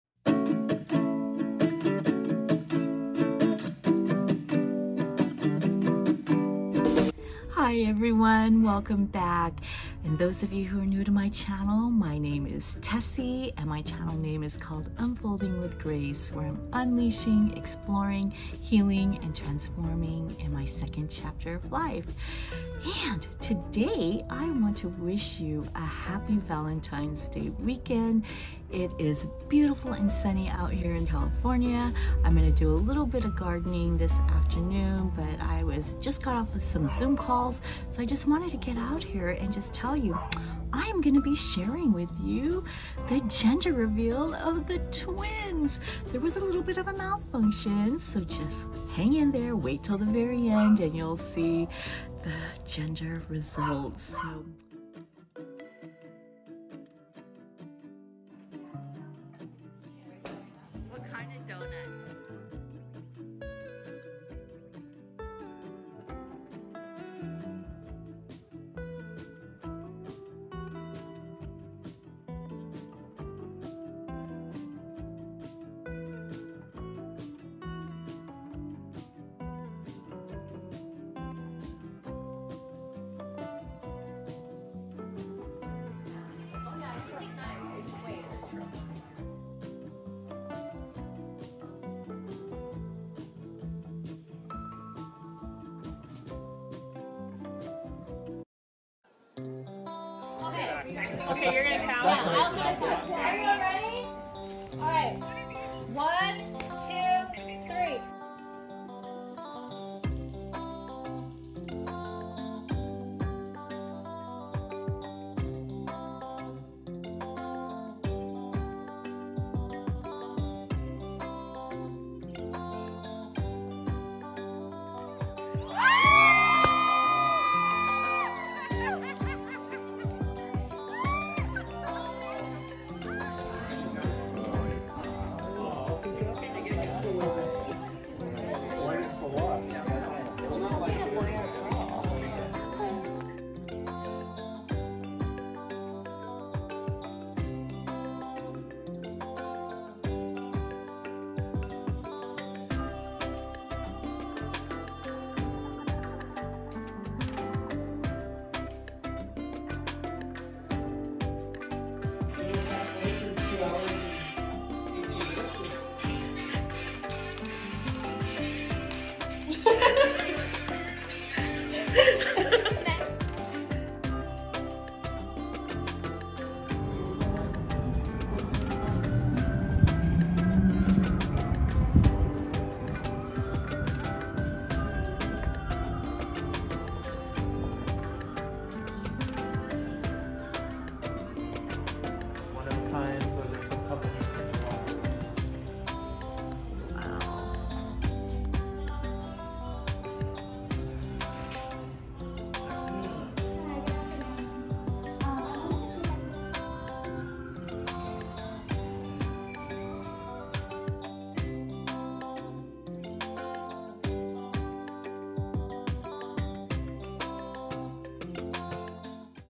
Twin Gender Reveal. Lola Tessie is excited for the arrival of her grandbabies!
Hi Friends,  I'm excited to share my grandbabies gender reveal.  It was an emotional and exciting day!  I'm already so in love with the little ones and I can't wait for their arrival!
In previous videos I mentioned that I am on a LOW BUY year so it will be interesting how I navigate staying on budget while wanting to buy the moon for these bundle of joy!
Enjoy the video!  Blessings to all,
Tessie